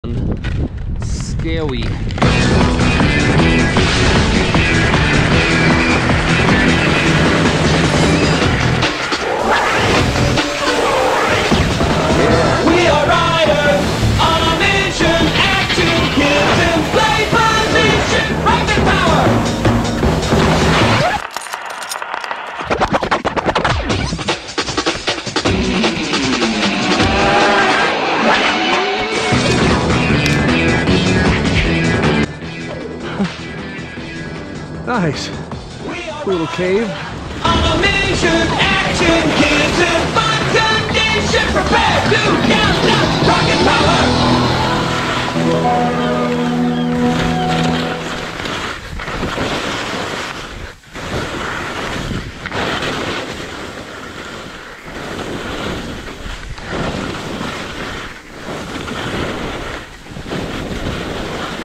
Grunge Couloir Ski Descent
1 of 2: Linked the Grunge Couloir and "Bury the Hatchet" near North Timpanogos

Avoided the corniced entrance to the Grunge and accidentally dropped into the wrong entrance. A short WI2 downclimb dropped me in the couloir proper where the snow was smooth and firm.

Music inspired by my childhood